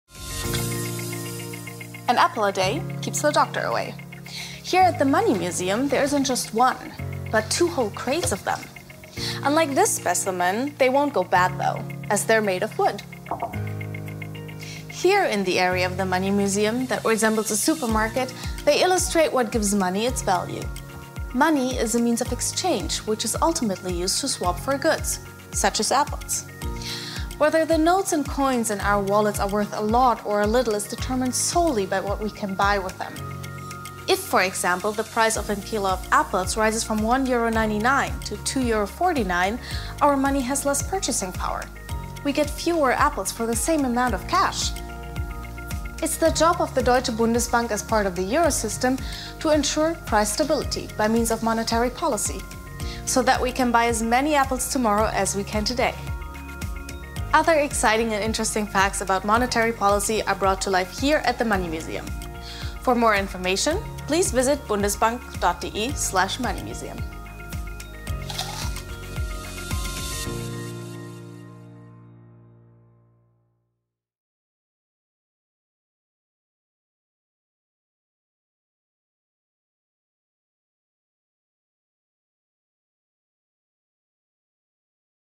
Goods for Money
Part of our money museum looks like a supermarket – though there aren’t actually any real groceries there, of course. In the second part of our video series, presenter Silviana Ursu shows what our money is worth and what price stability means.

Adress:
Deutsche Bundesbank
Wilhelm-Epstein-Straße 14
60431 Frankfurt am Main/Germany